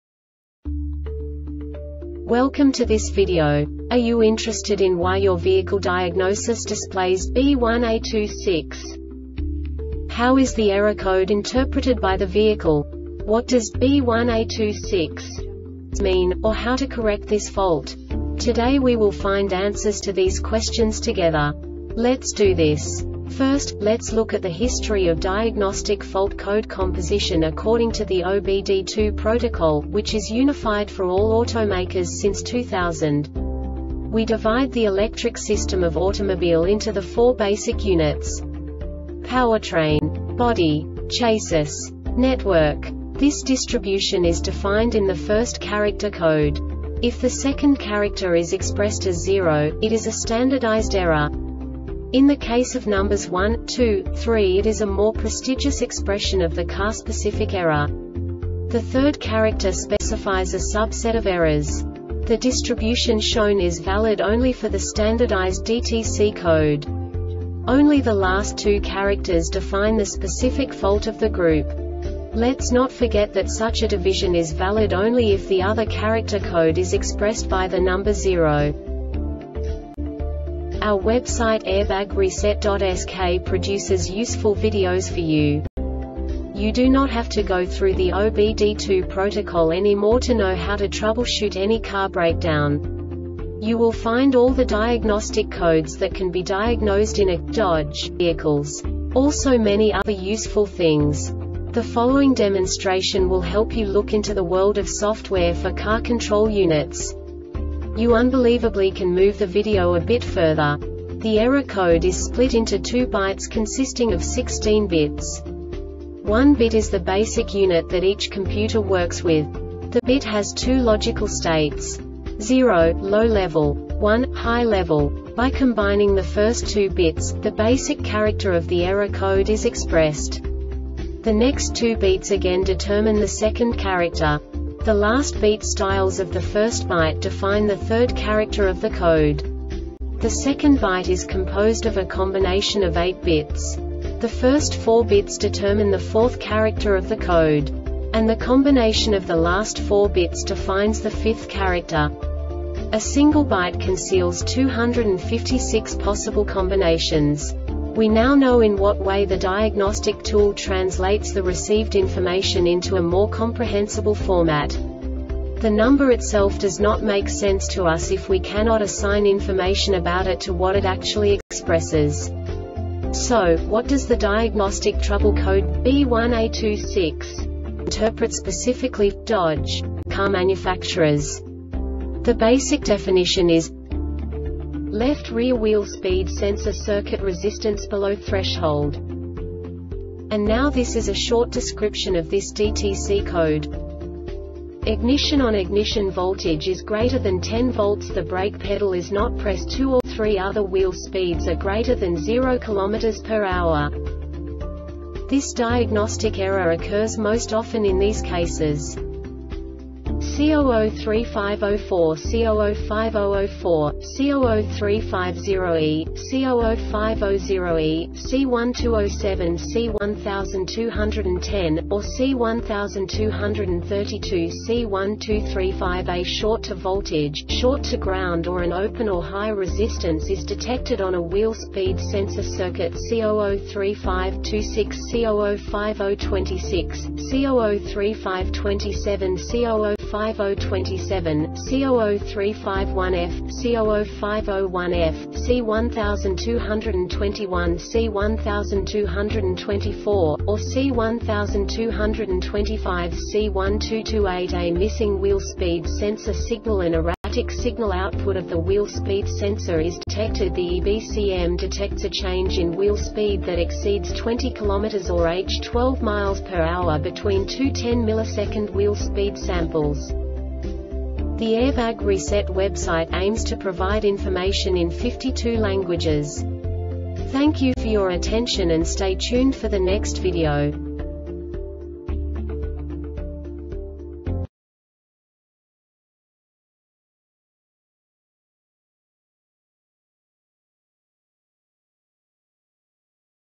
DTC Dodge B1A26 Short Explanation
Contents:
0:21 Basic DTC analysis according to OBD2 protocol standard.
1:48 Insight into programming
2:58 A diagnostically understandable description of the Diagnostic Trouble Code
3:05 Basic error definition

Make:
Dodge
Code:
B1A26
Definition:
Left Rear Wheel Speed Sensor Circuit Resistance Below Threshold
Description:
Cause:
C0035 04 - C0050 04, C0035 0E - C0050 0E,C1207 - C1210, or C1232 - C1235A short to voltage,short to ground or an open or high resistance is detected on a wheel speed sensor circuit.C0035 26 - C0050 26, C0035 27 - C0050 27, C0035 1F - C0050 1F,C1221 - C1224, or C1225 - C1228A missing wheel speed sensor signal.An erratic signal output of the wheel speed sensor is detected.The EBCM detects a change in wheel speed that exceeds 20 km or h (12 mph) between two 10 millisecond wheel speed samples.